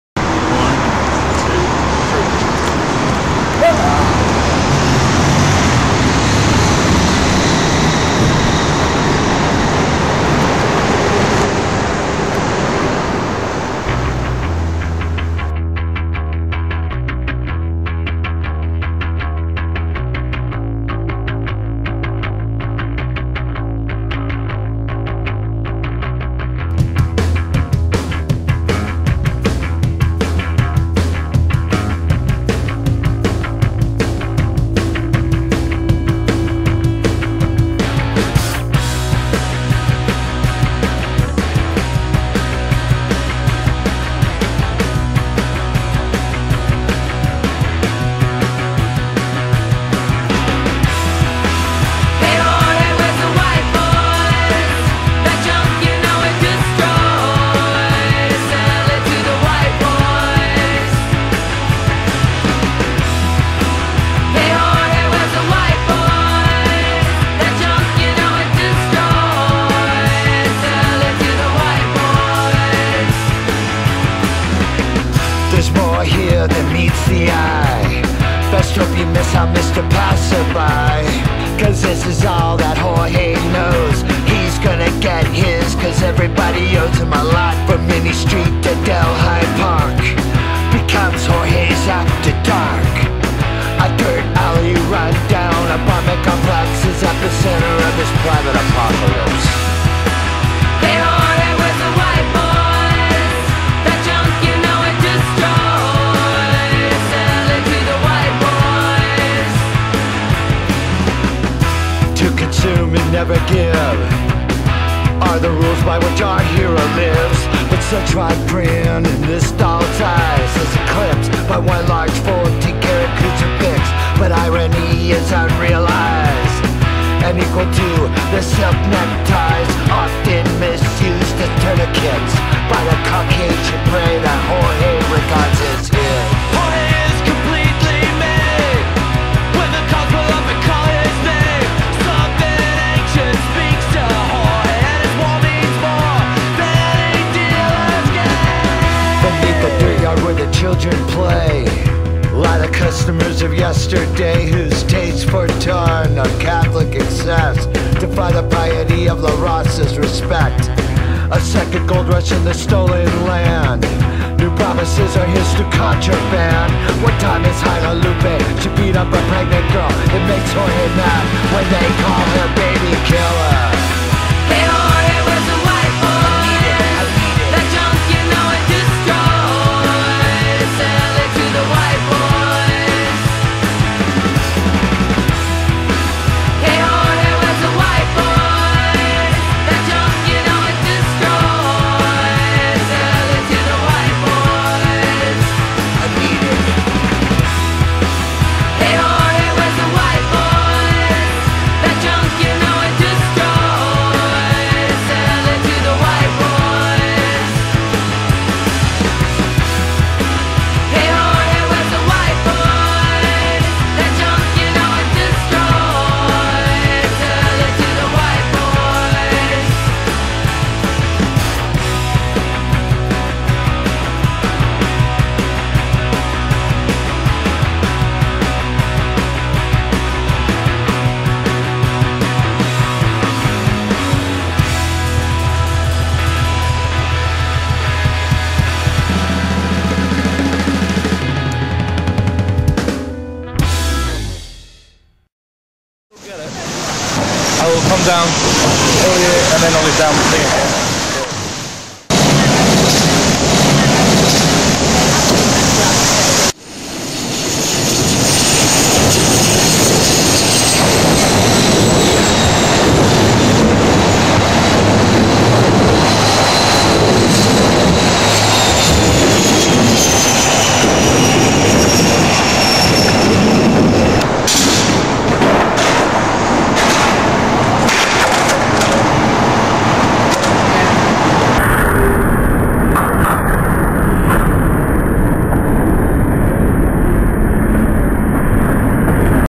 Death Ray Disorder
3 guys hit west london, 2 with skateboards, 1 with a camera. This is the result.